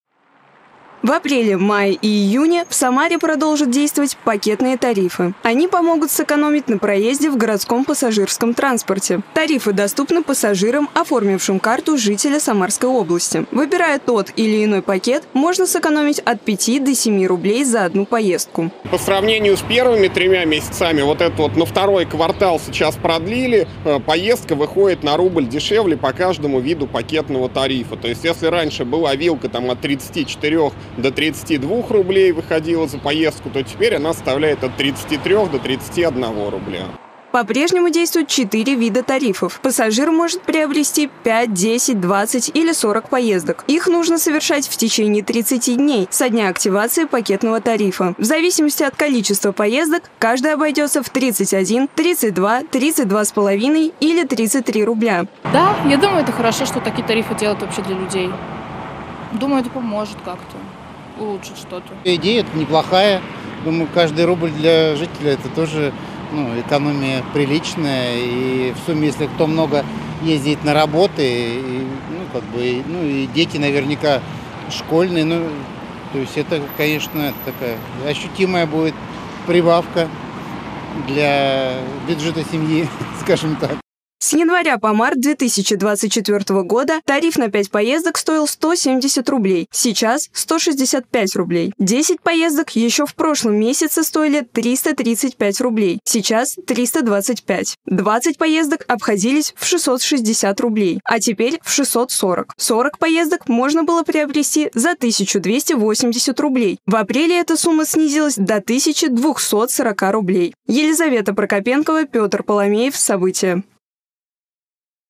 В Самаре продлили действие "пакетных" предложений по оплате проезда в общественном транспорте
С апреля по июнь в Самаре продолжат действовать "пакетные" тарифы на поездки в городском общественном транспорте. Существует 4 вида тарифов: на 5, 10, 20 и 40 поездок. При выборе одного из них  на каждой поездке можно будет сэкономить от 5 до 7 рублей. Выгоду посчитал наш корреспондент Елизавета Прокопенкова.